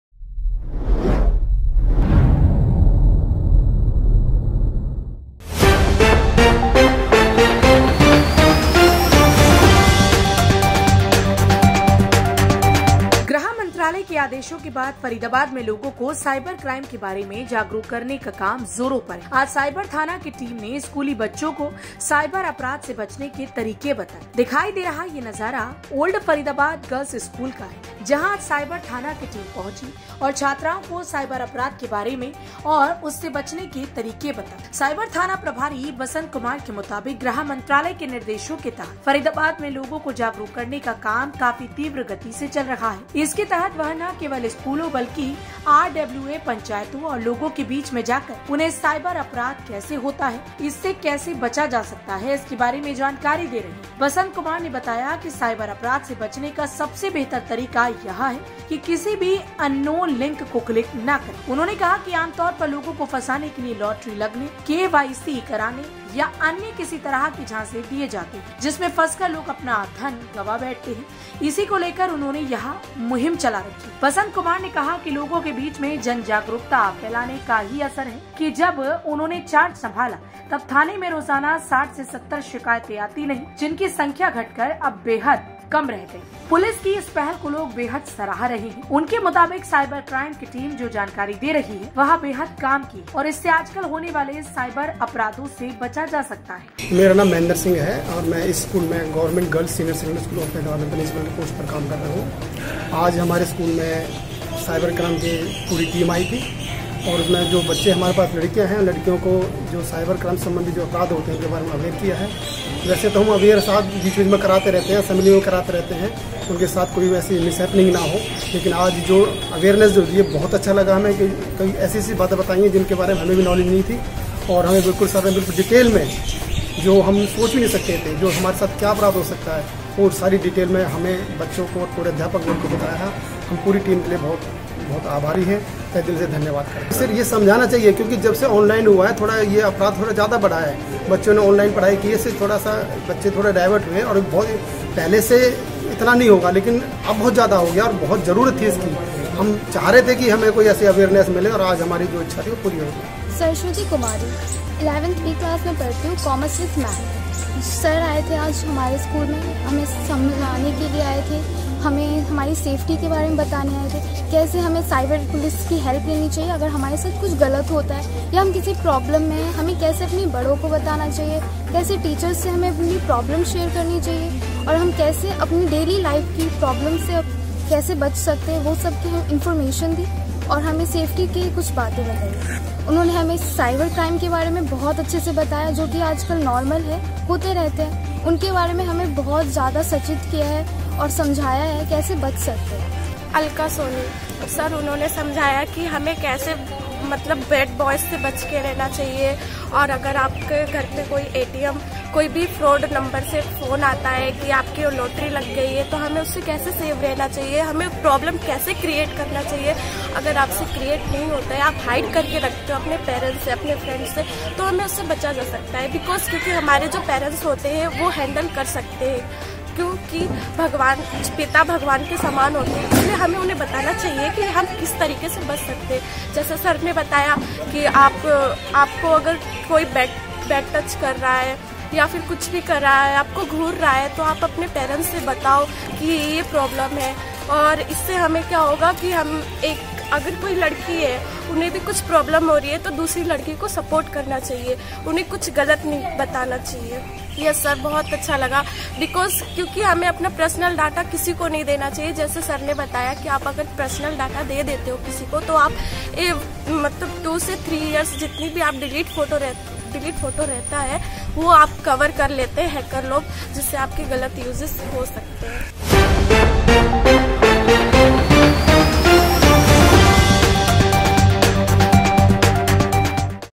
फरीदाबाद में लोगों को साइबर क्राइम के बारे में किया गया जागरूक
गृह मंत्रालय के आदेशों के बाद फरीदाबाद में लोगों को साइबर क्राइम के बारे में जागरूक करने का काम जोरों पर है । आज साइबर थाना की टीम ने स्कूली बच्चों को साइबर अपराध से बचने के तरीके बताएं ।